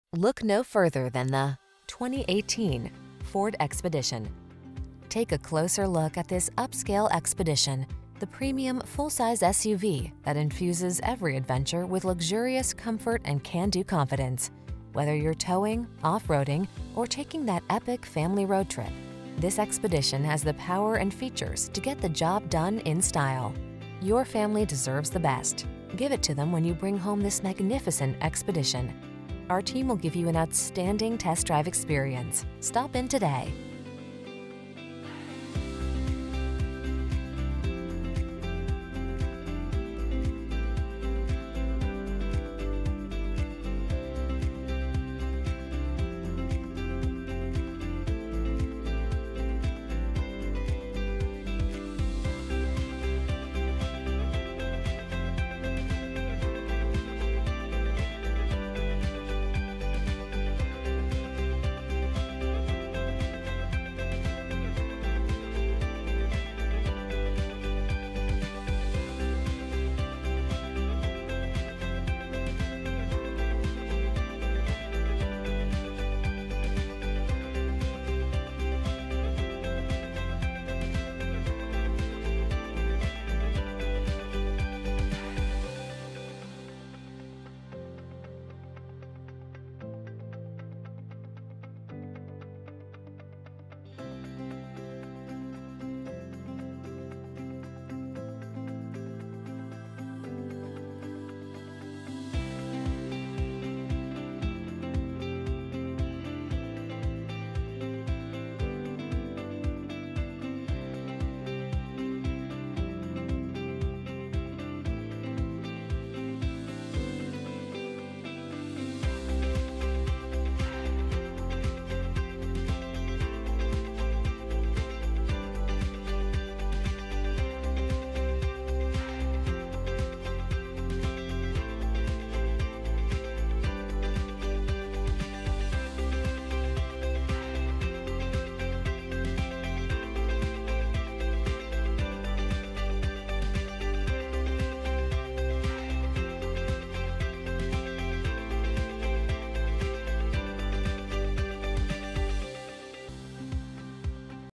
2018 Ford Expedition Riverside, Fontana, Redlands, Rancho Cucamonga, Palm Springs, CA 000P0914
Stone Gray Metallic Used 2018 Ford Expedition available in Riverside, California at Fairview Ford. Servicing the Fontana, Redlands, Rancho Cucamonga, Palm Springs, CA area.

Fairview Ford
292 North G St